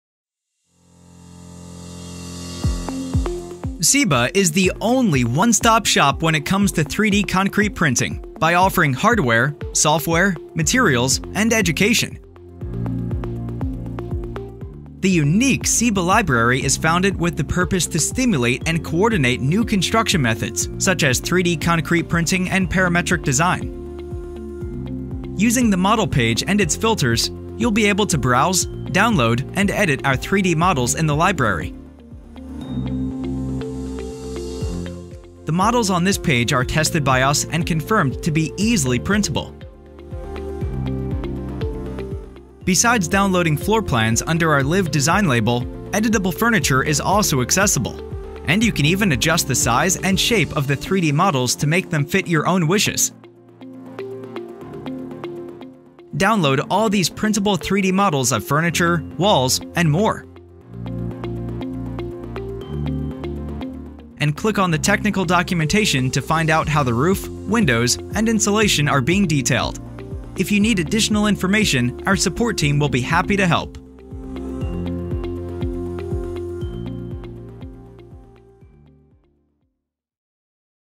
3D Printable and Editable Models by CYBE CONSTRUCTION
Our Lybrary is the digital platform where these 3Dcp models, hands-on experience and knowledge is shared to make 3Dcp more accessible for us and partners. This is done based on the four stage principles of the Lybrary; the Academy, Software, 3D Modes and the Knowledge base.

We believe that sharing is caring and therefore it is our ultimate goal, besides gathering information, to share it.